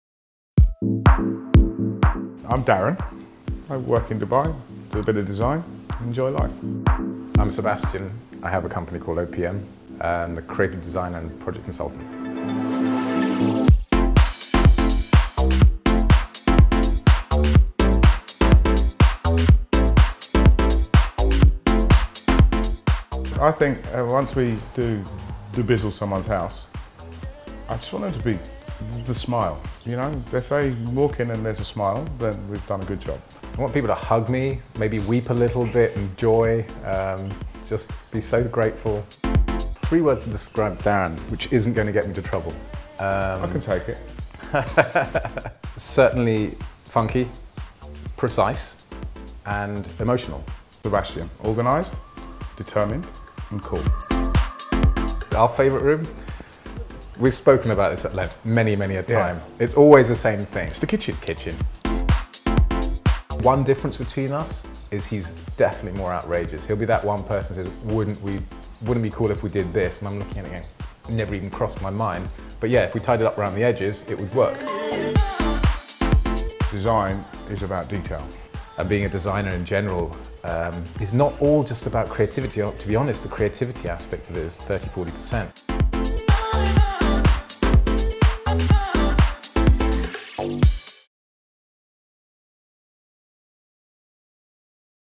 Dubizzle Your House - OPM Profile (shorter)
Two ordinary guys in one extraordinary city prepare to give one lucky dubbizler a home makeover with a difference. Let the adventure commence.